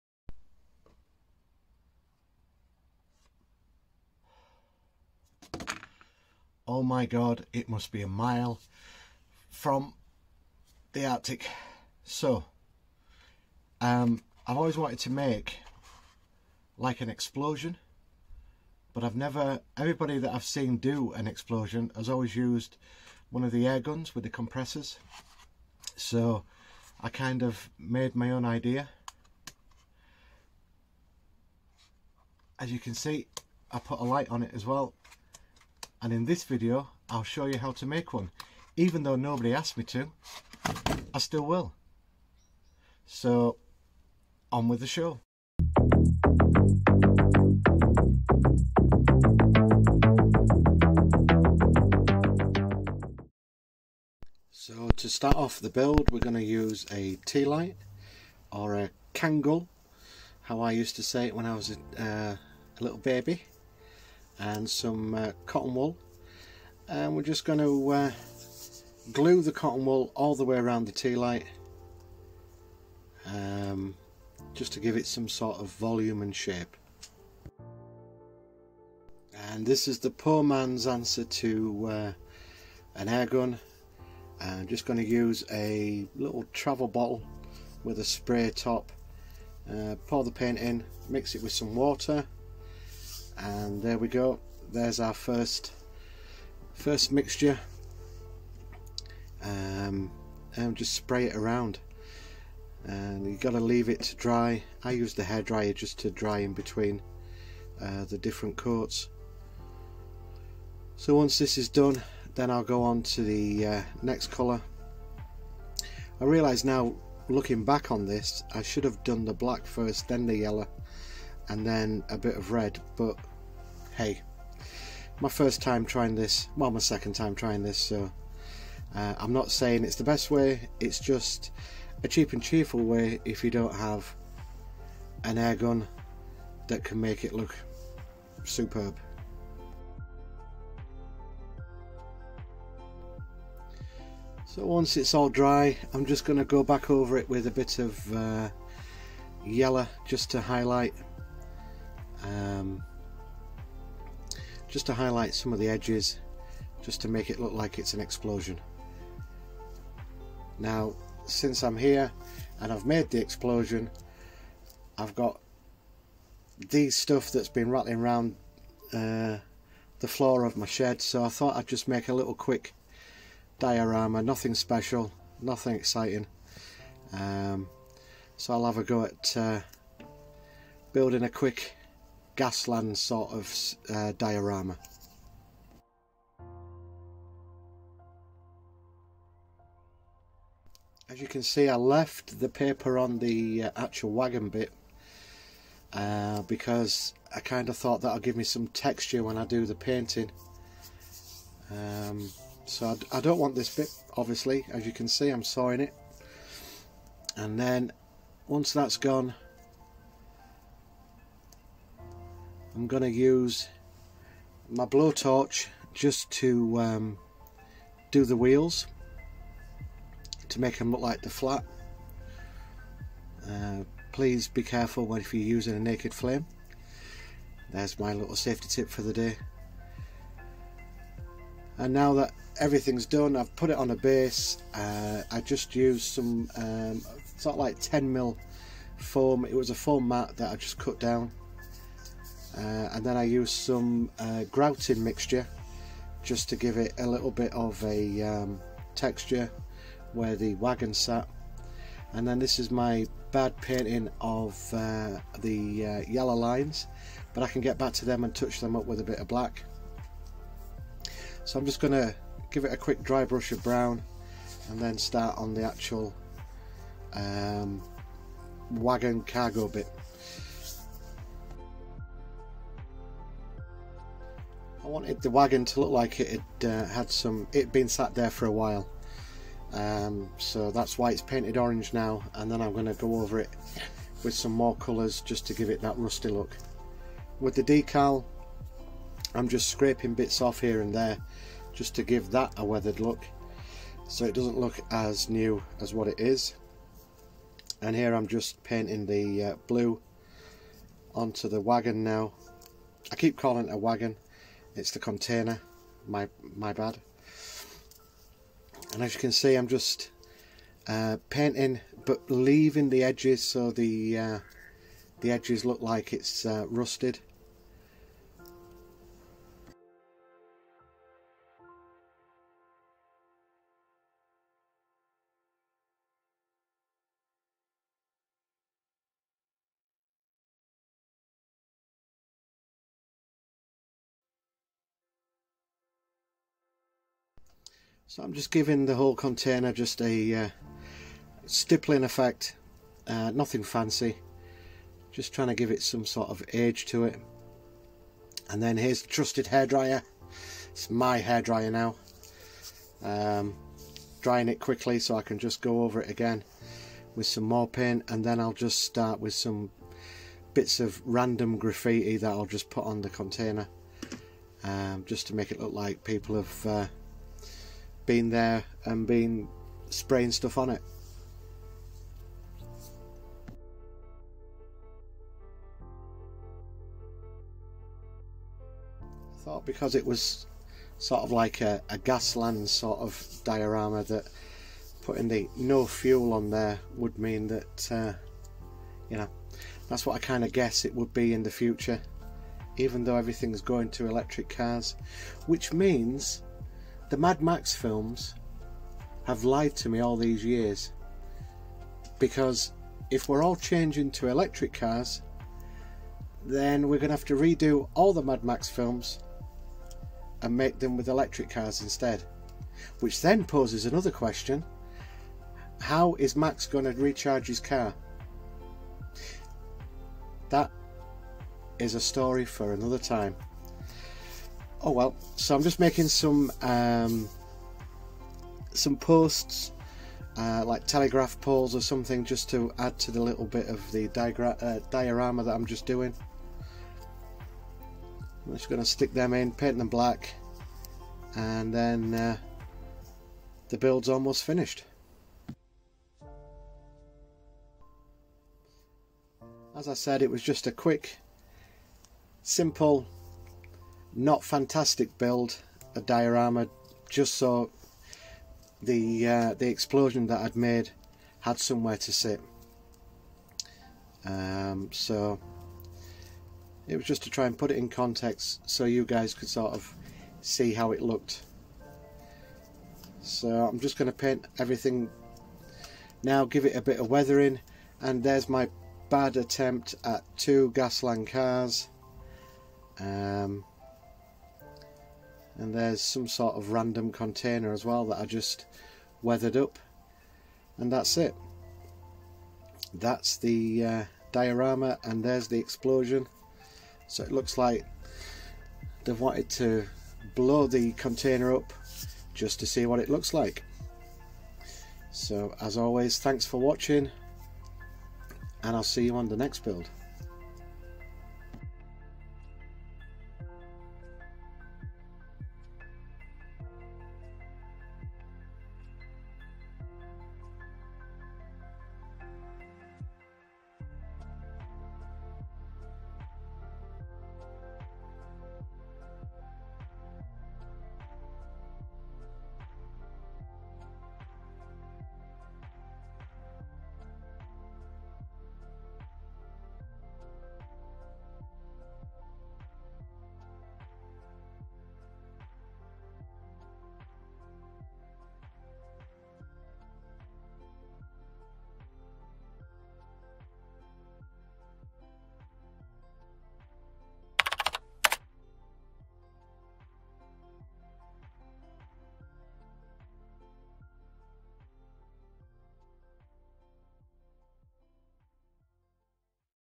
How to make a Gaslands Diorama. Ep15.
Ep15.  How to make a quick and easy explosion for your diorama if you don't own an airbrush.

I'd been racking my brain over how to make a quick and easy explosion for my diorama without having an airbrush and came up with a light bulb moment.
As always if you want to see me tackle a build or have any questions, ask me.